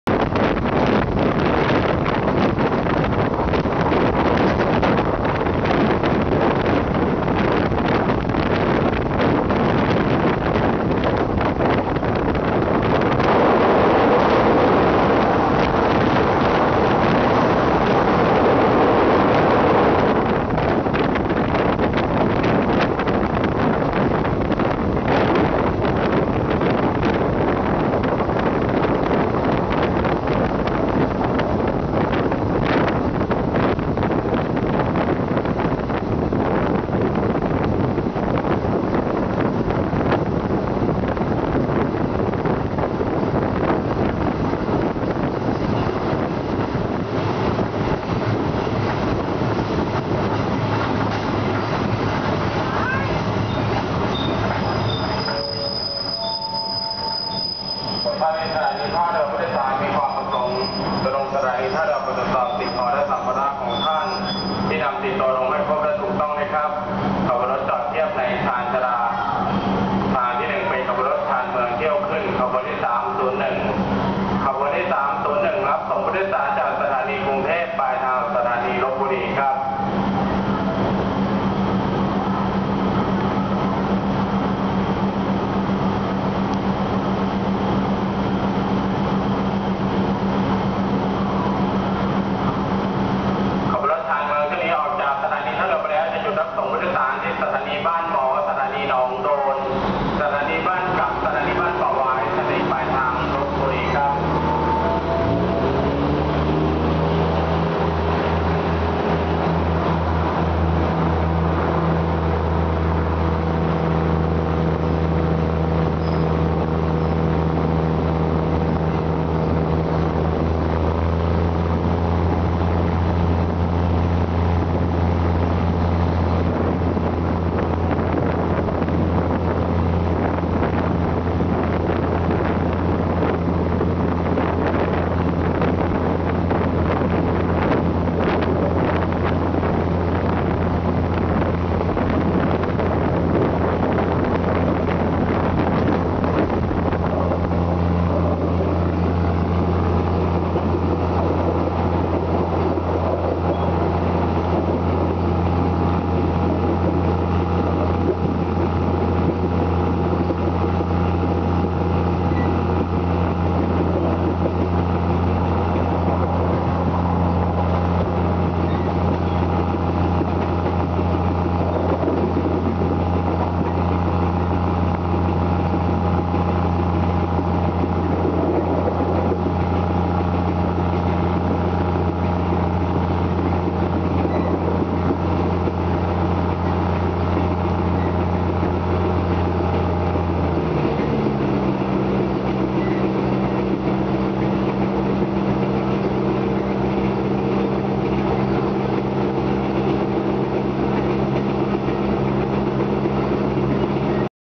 301 เข้าจอด สถานีท่าเรือ
รถชานเมืองที่ 301 กรุงเทพ-ลพบุรี เข้าจอด สถานีท่าเรือ  เมื่อวันที่ 5 ธันวาคม 2555